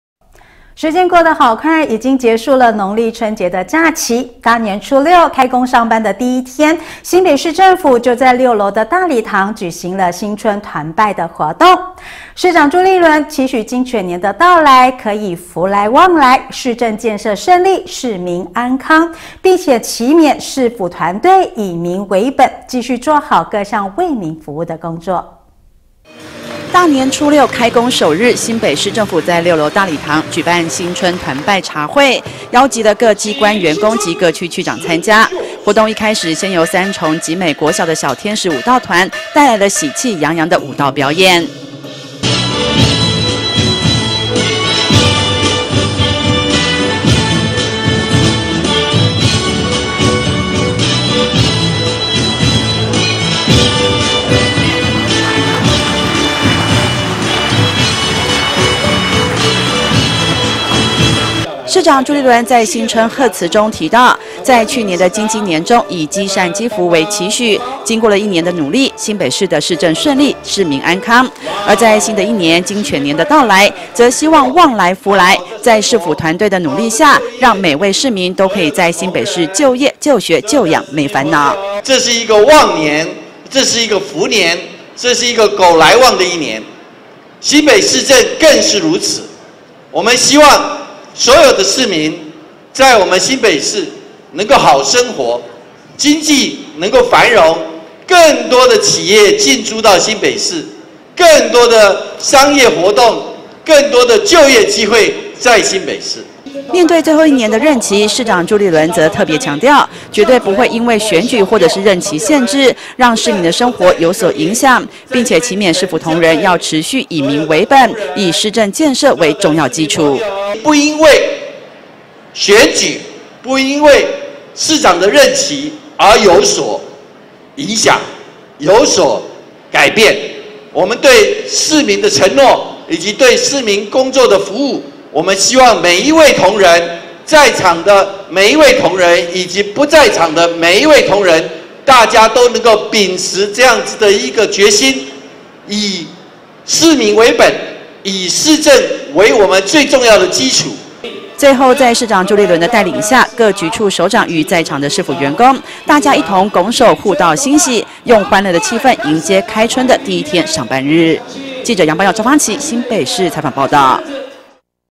1070221【大新店地方新聞】市府新春團拜 盼旺來福來庇新北
結束了農曆春節假期，大年初六，開工上班的第一天，新北市政府在六樓大禮堂舉行了新春團拜活動，市長朱立倫期許金犬年的到來，可以福來旺來，市政建設順利，市民安康，並期勉市府團隊”以民為本”繼續做好各項為民服務工作。